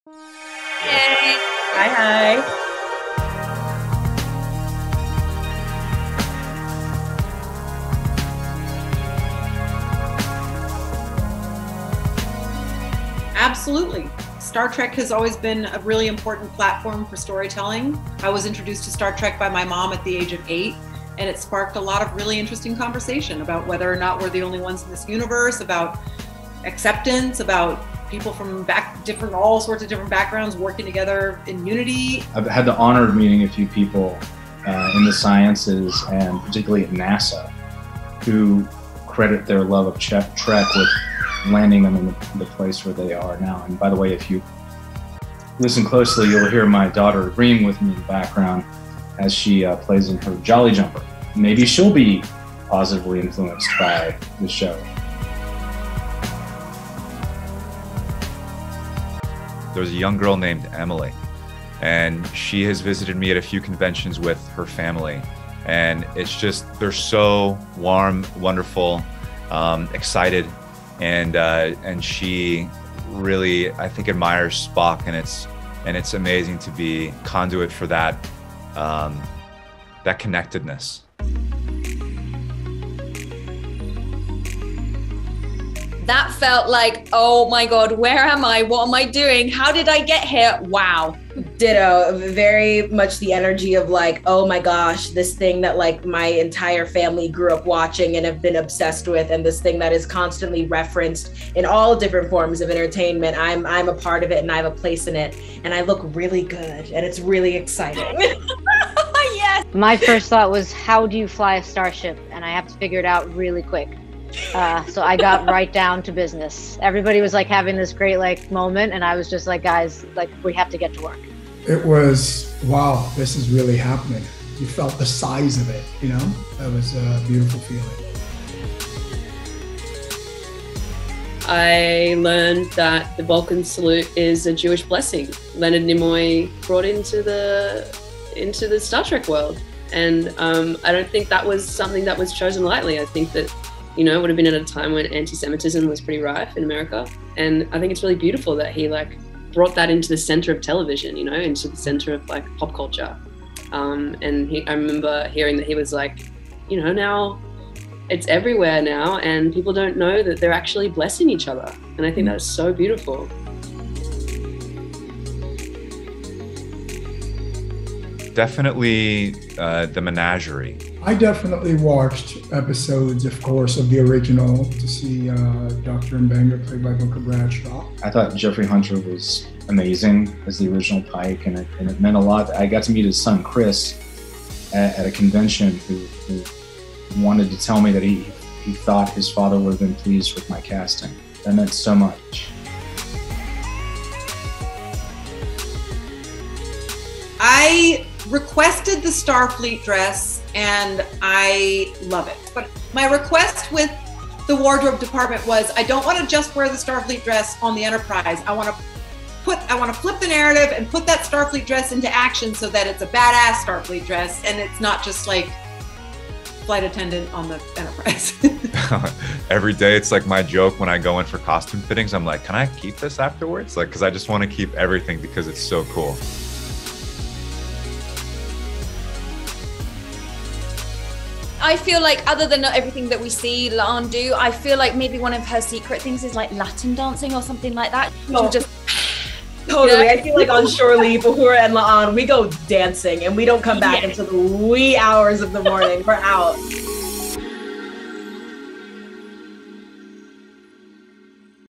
Star Trek: Strange New Worlds | Elenco responde perguntas dos fãs | Paramount Plus Brasil
Como se não bastasse ser um elenco maravilhoso e entregar uma série que nos deixa extasiados, os atores de StarTrek StrangeNewWorlds responderam perguntas dos fãs. Será que eles falaram de alguma curiosidade sua? Descubra assistindo o vídeo!